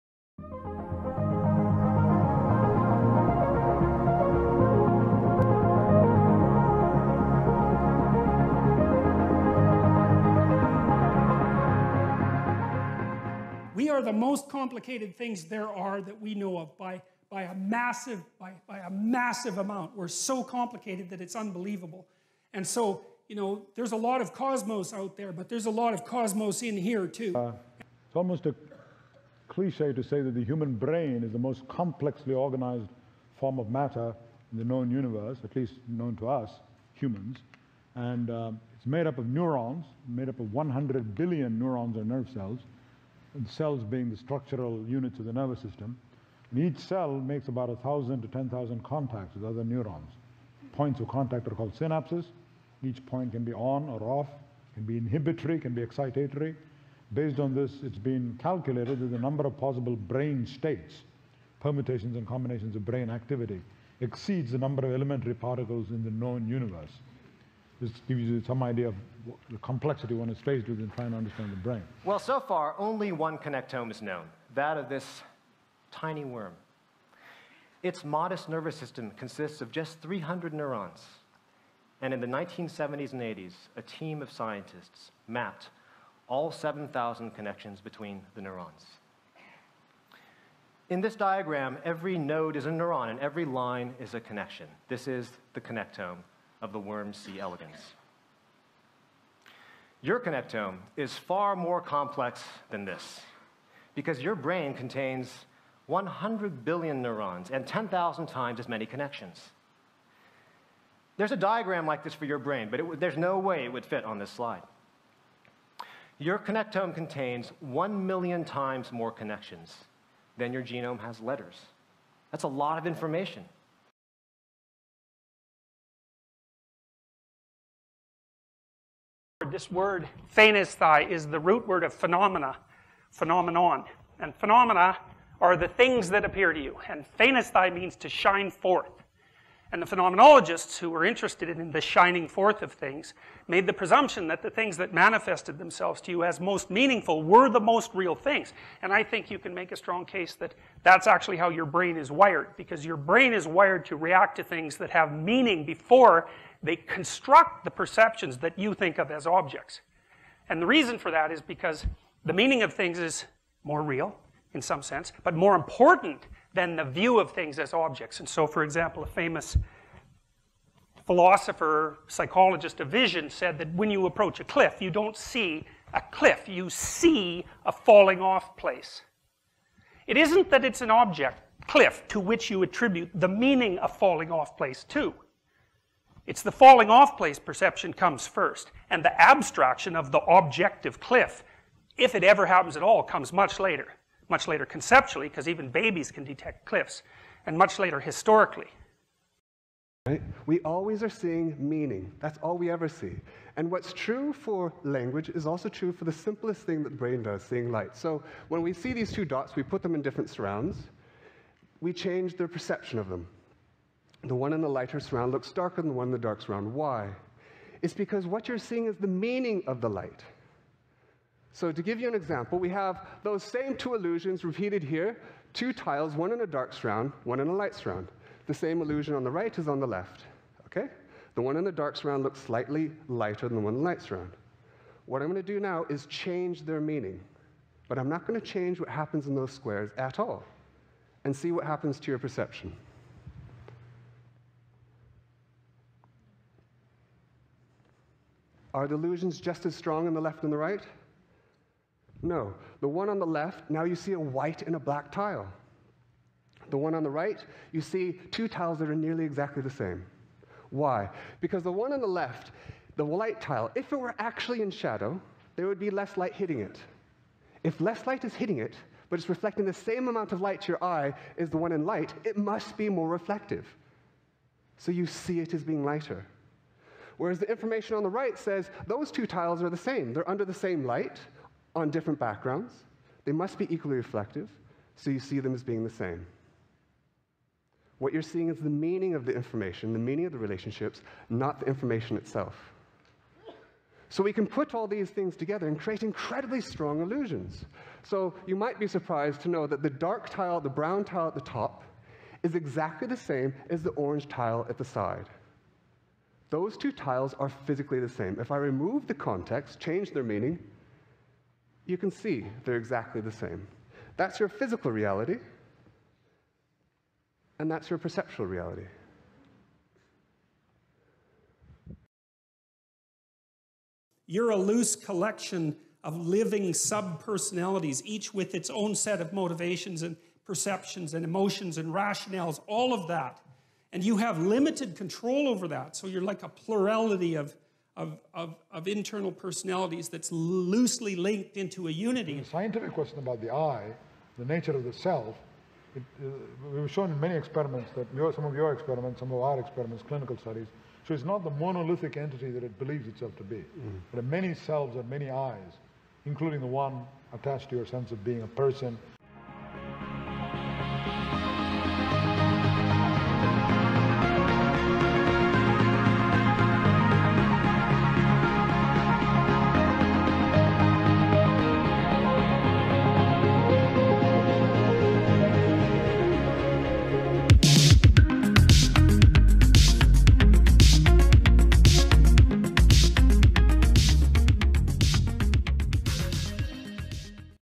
Jordan Peterson claims confirmed by expert neuroscientists!(GMTA5)
Legendary Neuroscientist V.S. Ramachandran with esteemed neuroscientist and artist Beau Lotto

Sebastian Seung Neuroscience and Physics, and head of the lab that brought us Eyewire!
Help map out the connectome! Play Eyewire and brag to your friends that you were busy doing neuroscience! ; )

Thanks to YouTube channels Philosophy Overdose, Tedx, Electronic Gems as well!